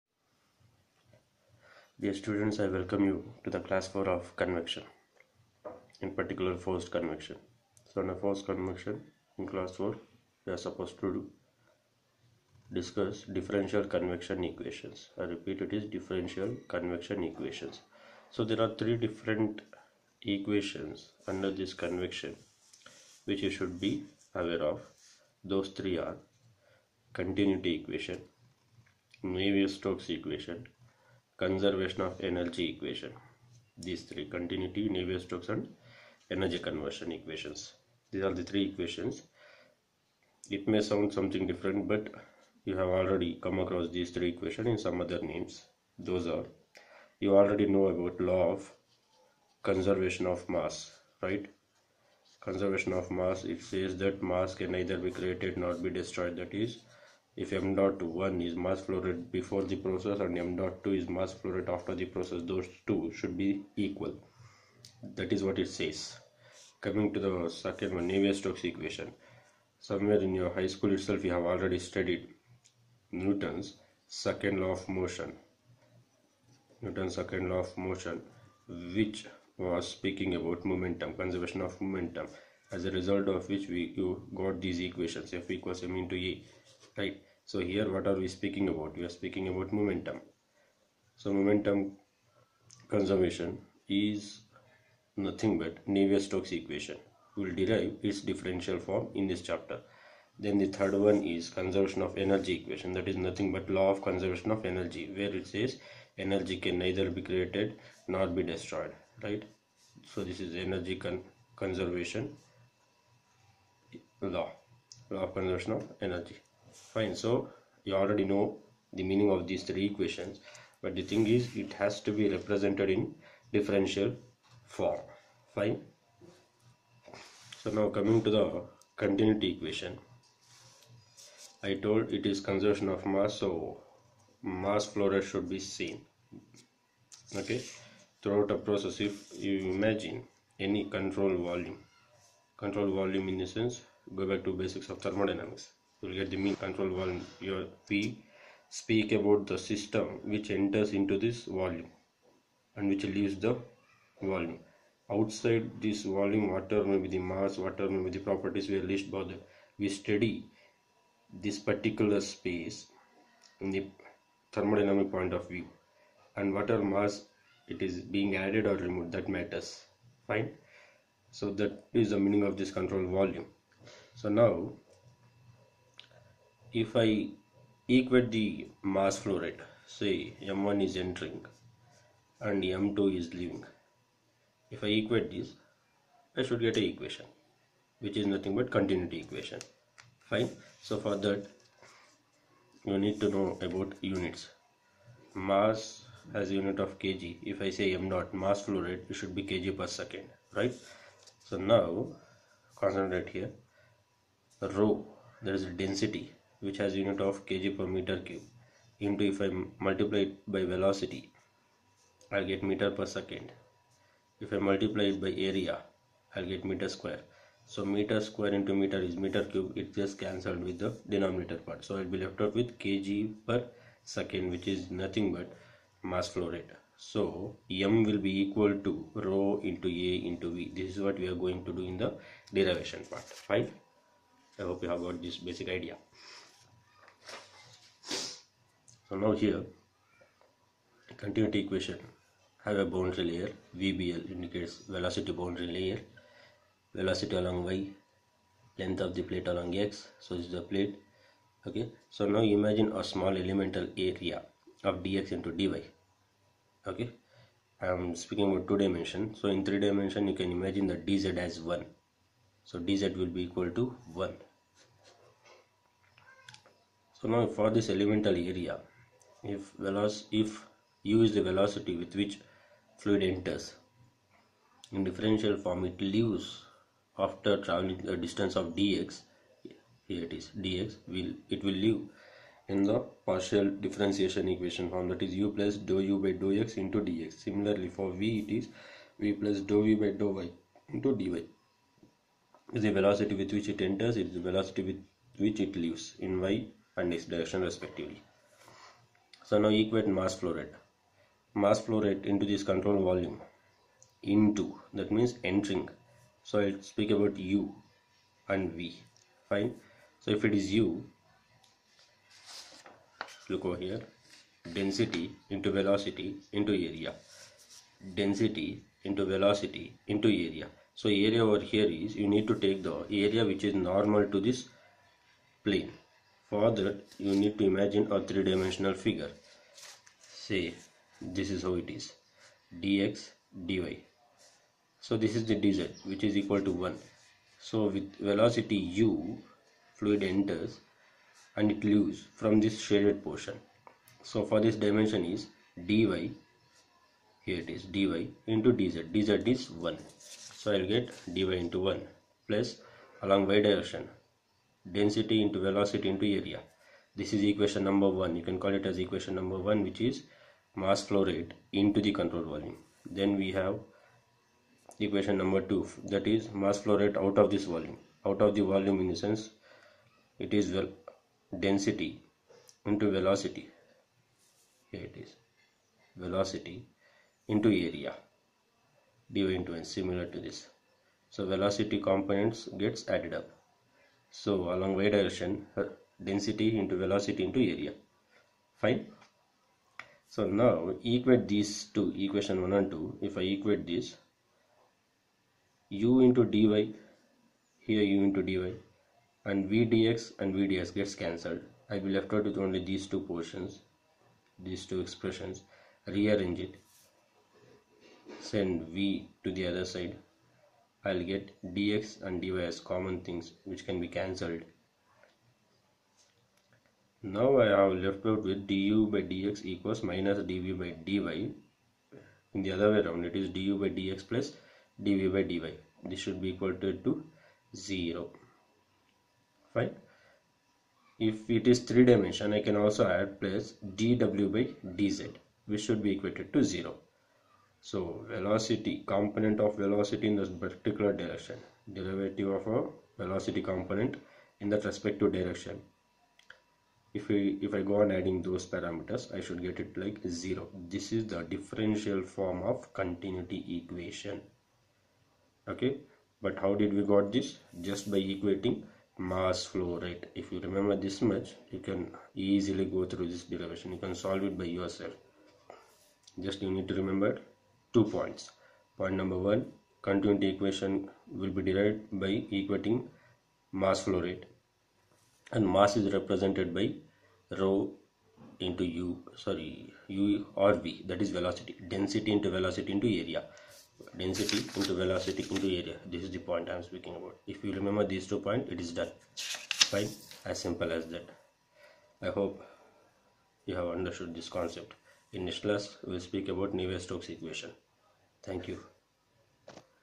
Convection - Continuity Equation (part 4)
In this video differential form of one of convection equation has been explained along with derivation.